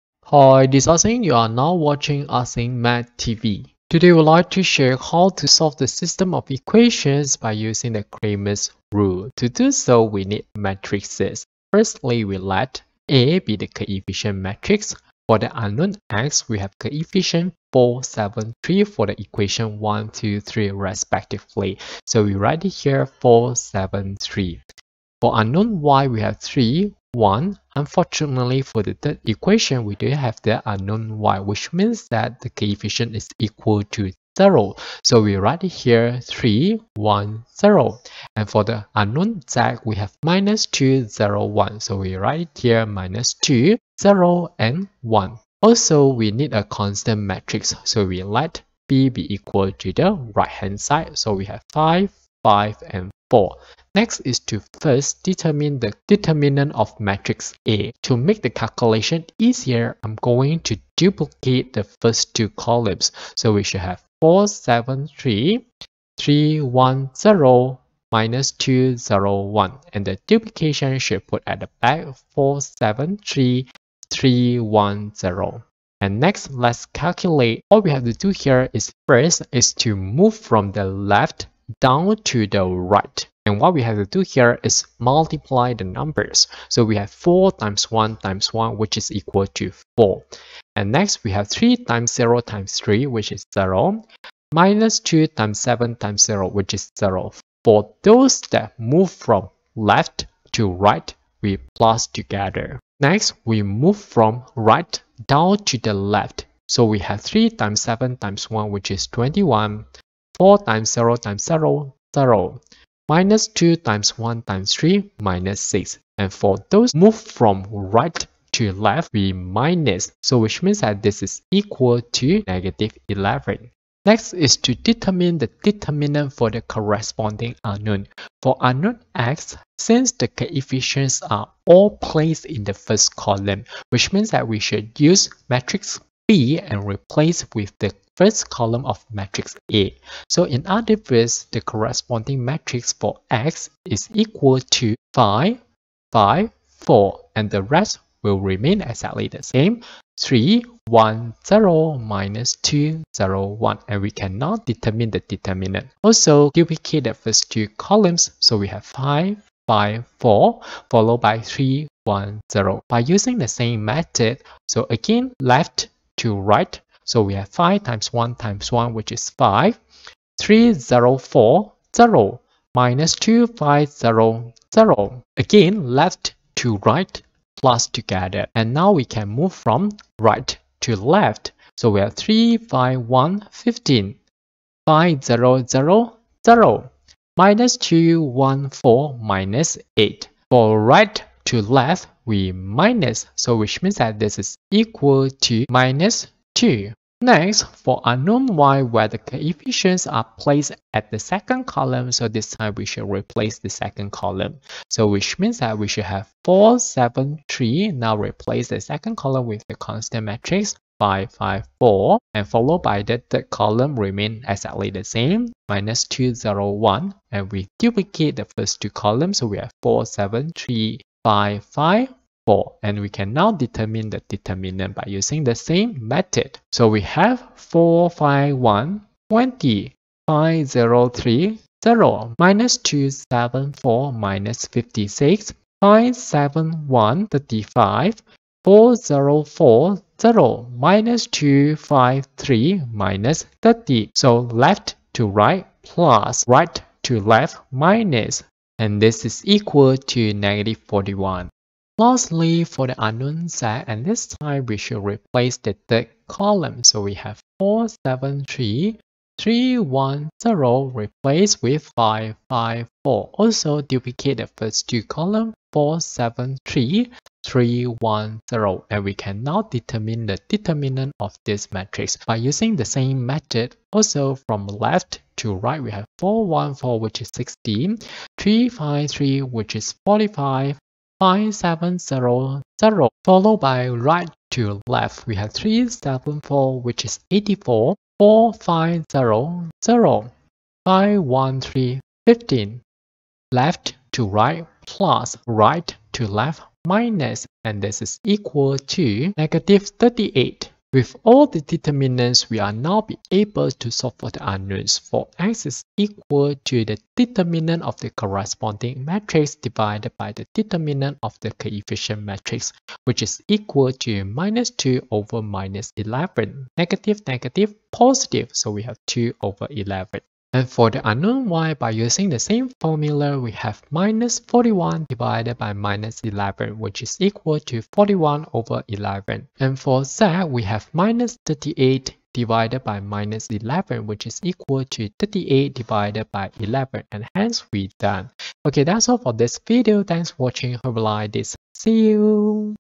Solve using Cramer's rule 4x+3y-2z=7x+y=5 3x+z=4
Solve the following system of equations by using Cramer's rule 4x+3y-2z=7x+y=5 3x+z=4.
Solve the equations by Cramer's rule 4x+3y-2z=7x+y=5 3x+z=4 .
Solve the system of equations in three variables (unknowns) by using the Cramer's rule.